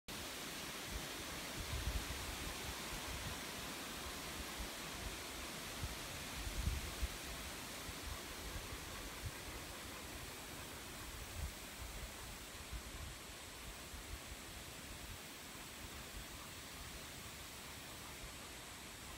An Orange-spotted Purple up in an Oak Tree
What's unusual about this orange-spotted purple is that it has its wings spread while resting in the tree. Usually they would be folded.